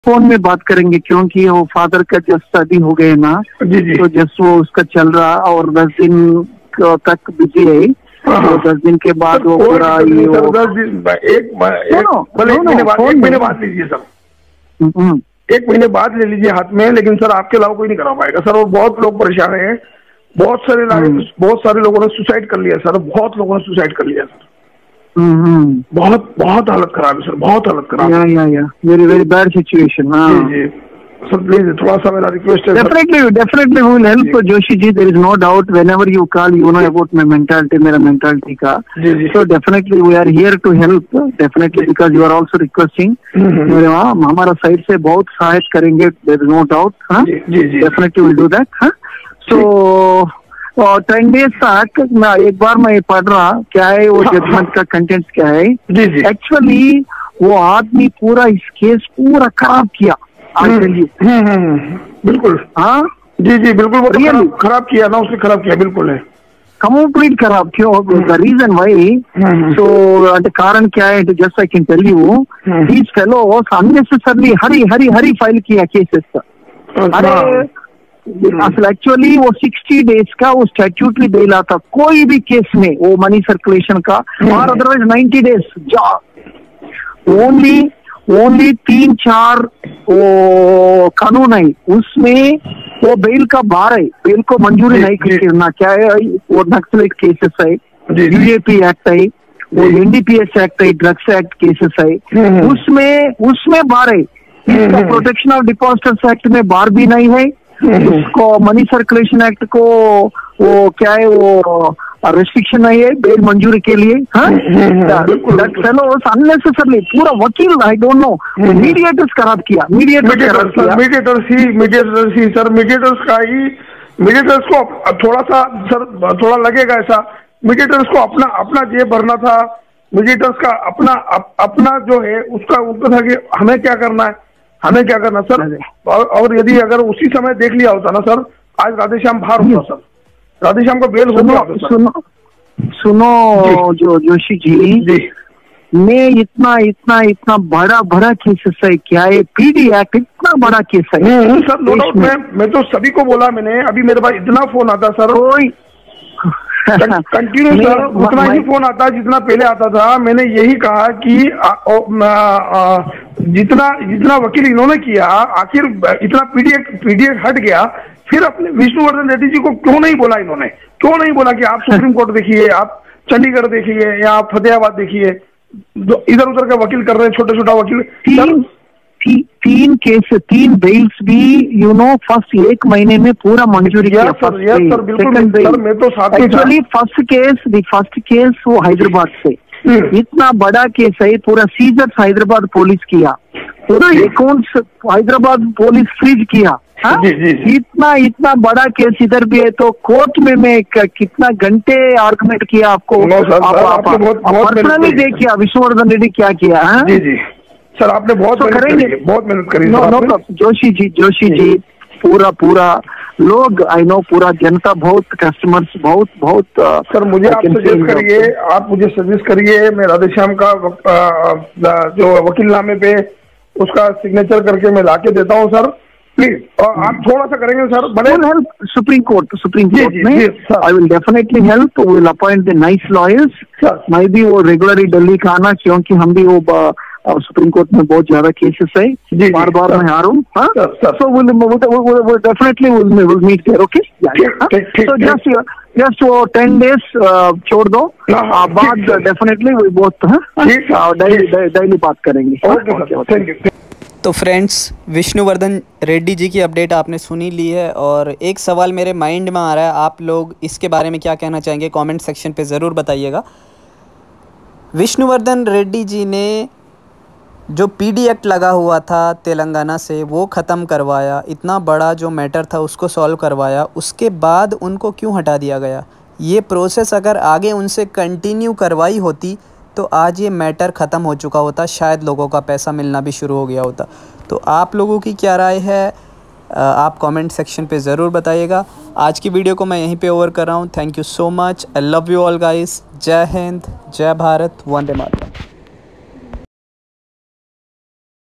Vishnu Vardhan Reddy ji ke future maker ko leke Latest update|29-09-2020|ख़ुशख़बरी फ़्यूचर मेकर
दोस्तों नमस्कार स्वागत है आपका नए वीडियो में इस वीडियो में मैंने आपको विष्णुवर्धन रेड्डी जी की लेटेस्ट अपडेट से अवगत कराया है कुछ आशा लग रही है कि लोगों का पैसा उन तक आ सकता है.

आशा करता हूँ वीडियो आपको पसंद आएगी.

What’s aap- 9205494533